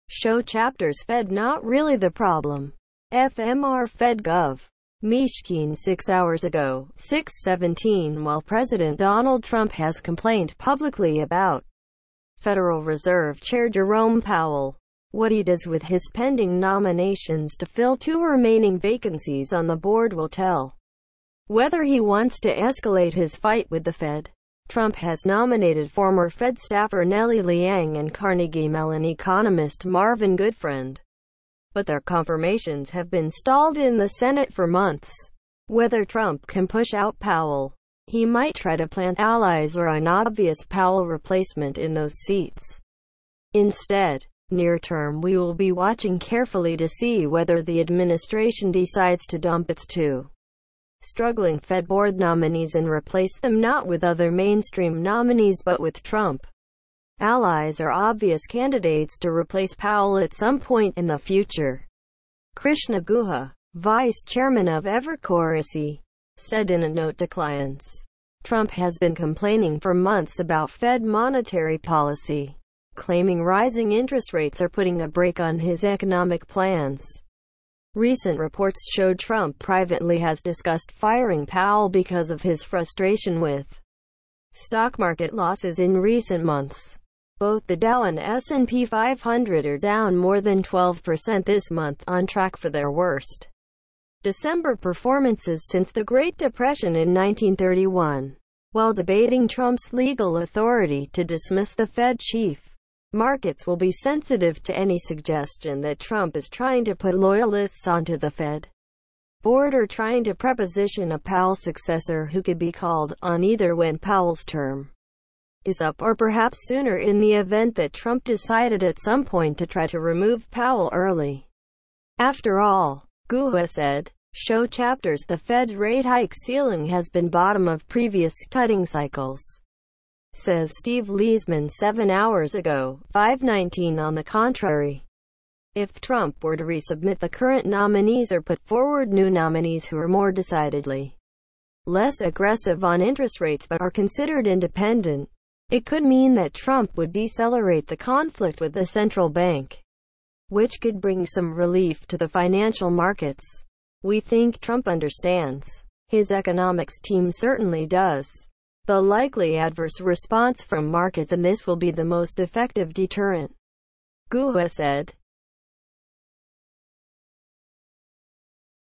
Look to the two remaining Fed board nominees for clues about Trump-Powell battle
show chapters                                       Fed not really the problem: Fmr. Fed Gov. Mishkin                              6 Hours Ago |  06:17 While President Donald Trump has complained publicly about Federal Reserve Chair Jerome Powell, what he does with his pending nominations to fill two remaining vacancies on the board will tell whether he wants to escalate his fight with the Fed. Trump has nominated former Fed staffer Nellie Liang and Carnegie Mellon economist Marvin Goodfriend, but their confirmations have been stalled in the Senate for months. Whether Trump can push out Powell, he might try to plant allies or an obvious Powell replacement in those seats, instead. Near term we will be watching carefully to see whether the administration decides to dump its two struggling Fed Board nominees and replace them not with other mainstream nominees but with Trump allies or obvious candidates to replace Powell at some point in the future, Krishna Guha, vice chairman of Evercore ISI, said in a note to clients. Trump has been complaining for months about Fed monetary policy, claiming rising interest rates are putting a brake on his economic plans.  Recent reports  showed Trump privately has discussed firing Powell because of his frustration with stock market losses in recent months. Both the Dow and S&P 500 are down more than 12 percent this month -- on track for their  worst December performances since the Great Depression  in 1931. While debating Trumps legal authority to dismiss the Fed chief, markets will be sensitive to any suggestion that Trump is trying to put loyalists onto the Fed Board or trying to preposition a Powell successor who could be called on either when Powells term is up or perhaps sooner in the event that Trump decided at some point to try to remove Powell early after all, Guha said. show chapters                                       The Feds rate hike ceiling has been bottom of previous cutting cycles, says Steve Liesman                              7 Hours Ago |  05:19 On the contrary, if Trump were to resubmit the current nominees or put forward new nominees who are more decidedly less aggressive on interest rates but are considered independent, it could mean that Trump would decelerate the conflict with the central bank, which could bring some relief to the financial markets. We think Trump understands  his economics team certainly does  the likely adverse response from markets and this will be the most effective deterrent, Guha said.